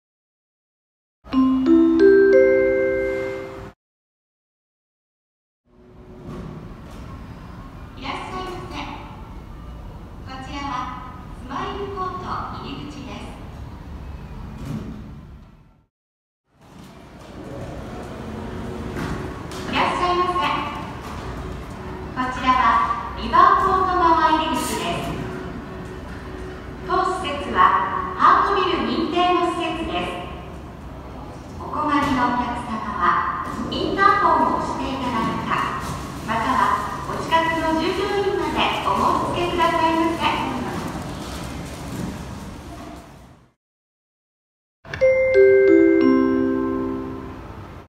【2種類の声】イオンモール広島祇園 入口案内放送
放送は草津と一緒？
左右側の入口は①番のように簡素な放送ですが、メイン(中央)の入口になると車椅子貸出放送が流れます。
放送①…0:06
放送②…0:17

この放送は各種案内放送に載っています。